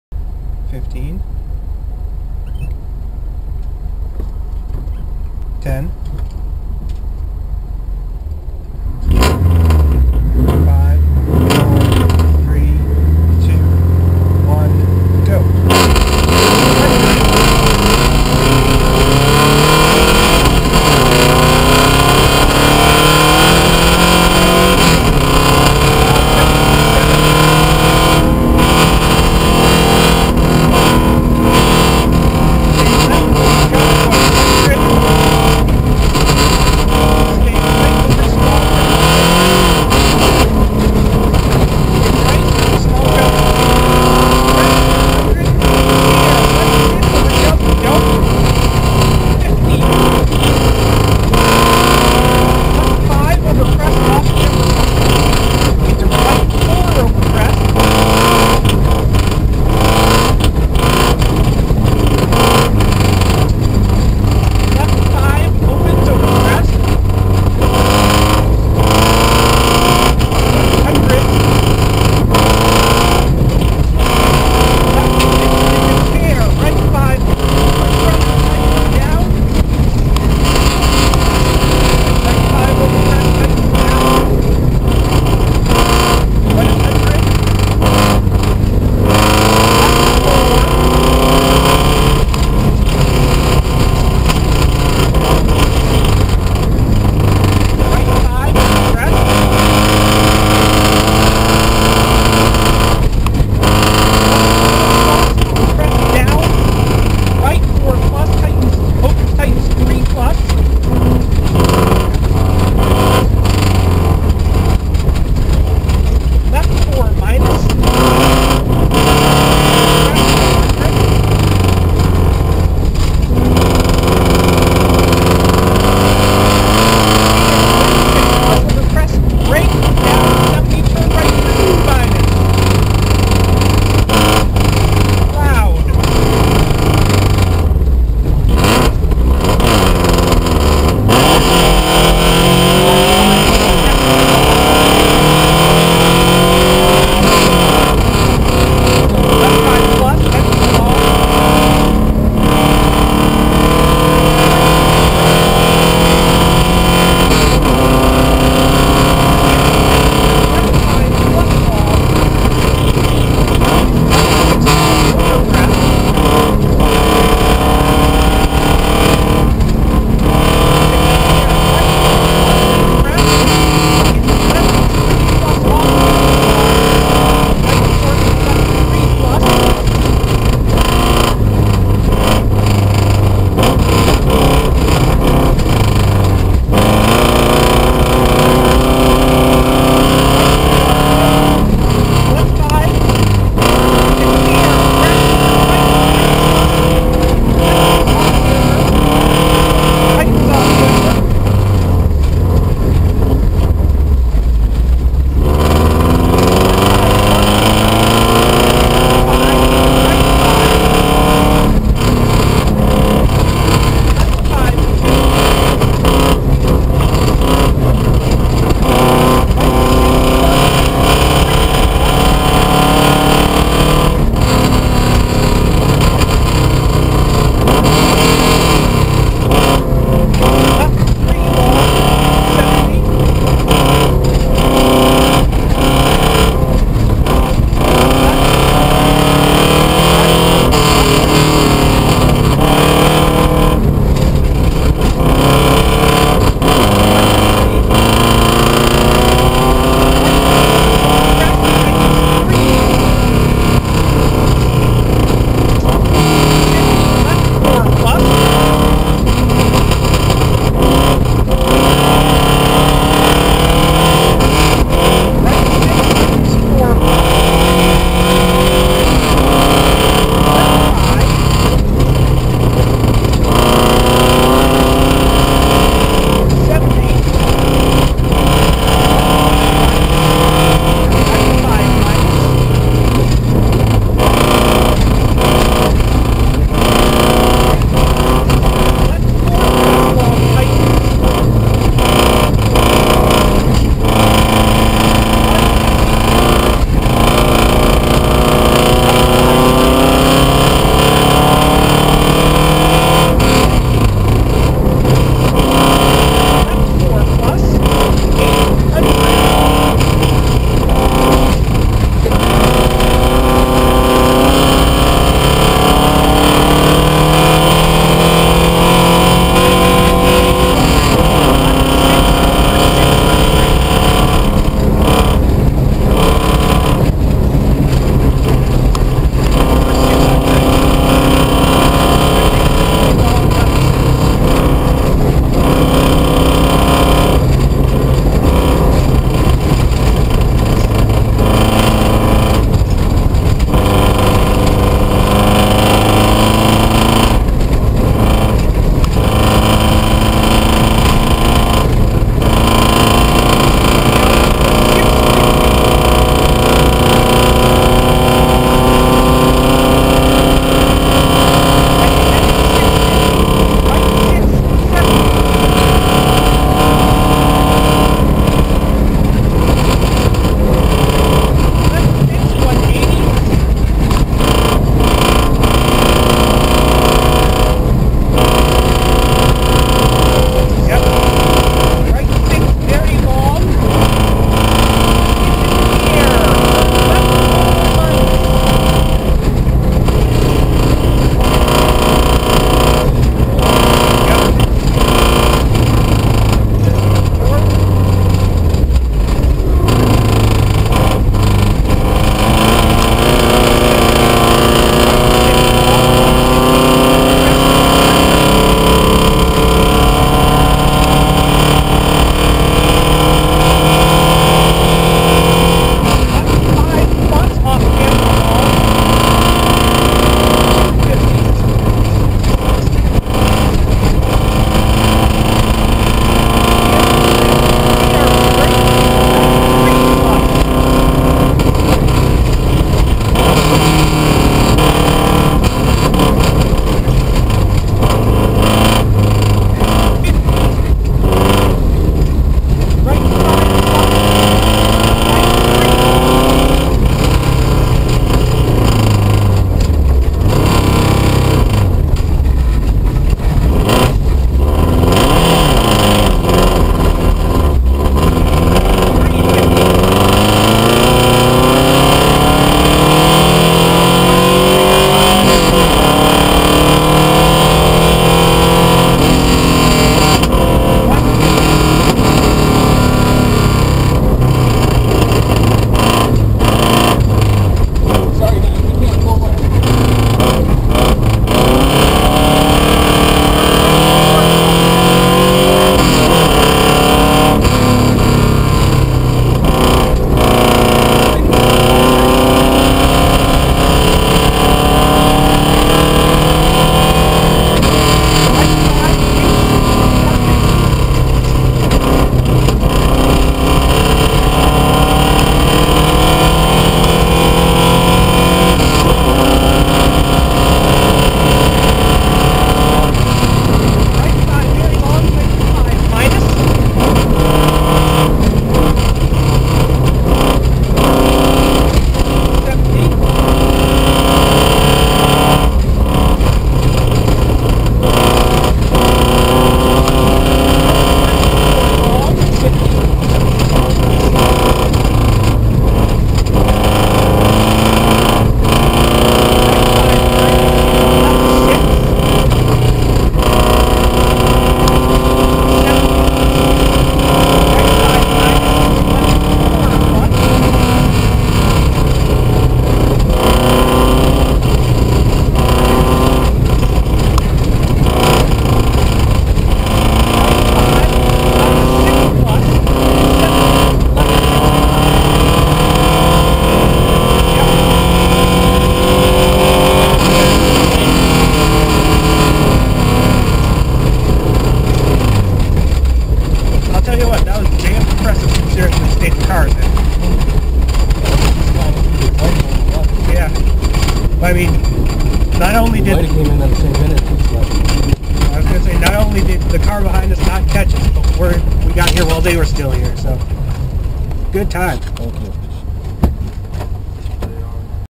Sno*Drift 2015 SS11 - Wilcox/Harrell
In car from the River Valley Rally Subaru Impreza Outback of Ryan Wilcox & Steven Harrell on the Old State - Huff stage of the 2015 Sno*Drift Rally in Atlanta Michigan.